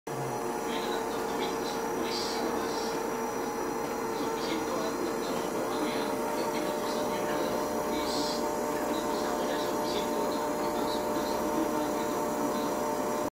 DeLonghi II Gelataio Ice Cream Maker
Testing our new Ice Cream maker from Delonghi.